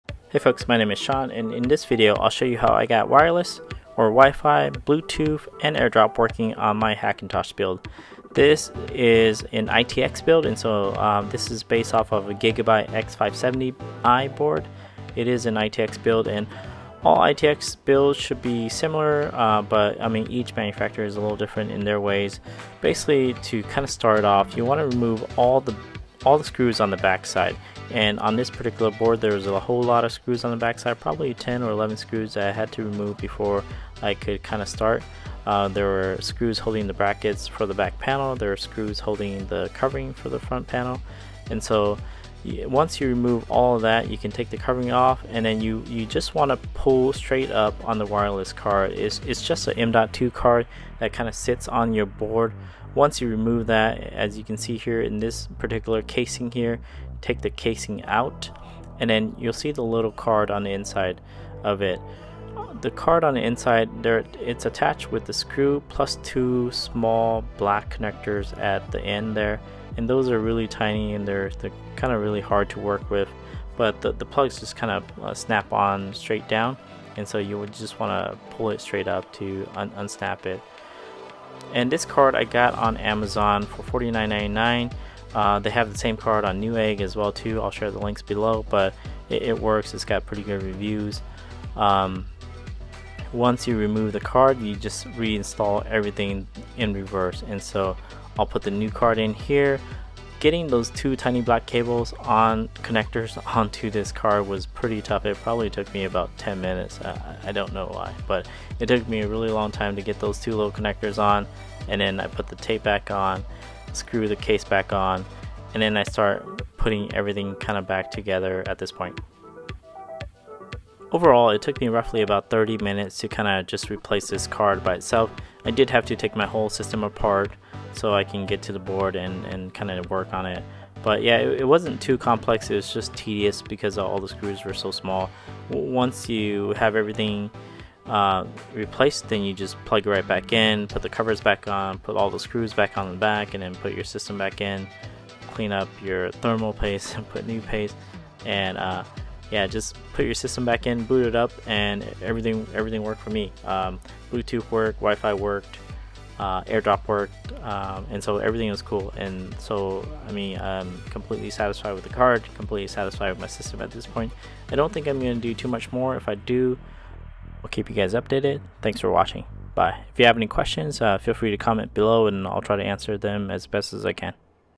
Hackintosh ITX WiFi Bluetooth AirDrop Card Replacement Catalina macOS Success
This video is about Hackintosh ITX WiFi Bluetooth AirDrop Card Replacement Catalina macOS

This is the link to the wifi card.